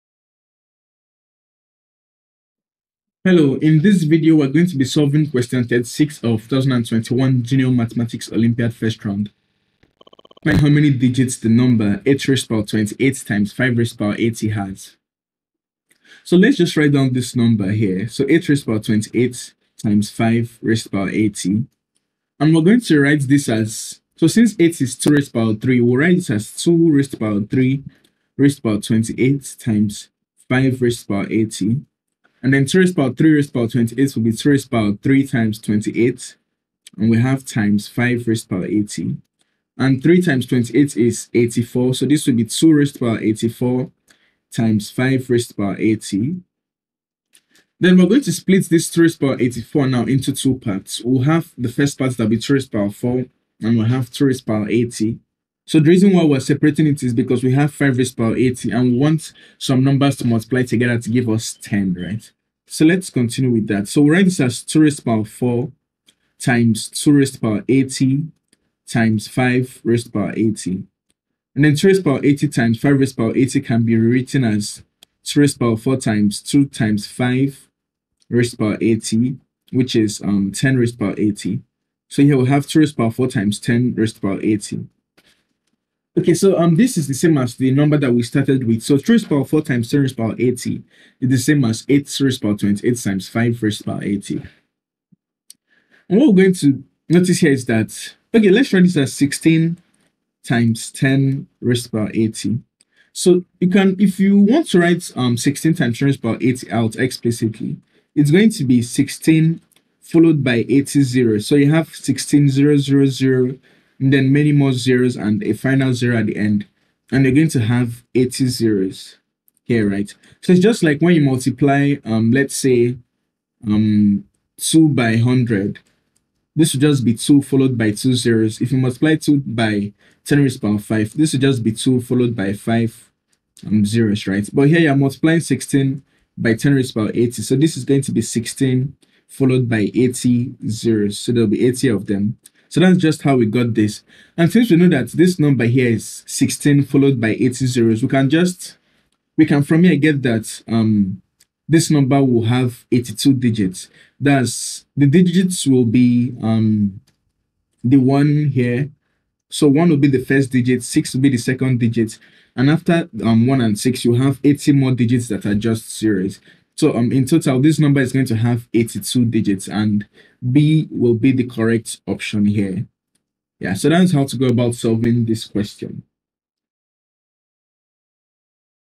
2021/2022 NMC Nigerian Mathematics Olympiad 1st Round Junior P36
This is the solution to Problem 36 of the Nigeria Mathematics Olympiad 1st Round 2021/2022 Junior Category by one of our instructors, Okezie Chiedozie, at Special Maths Academy.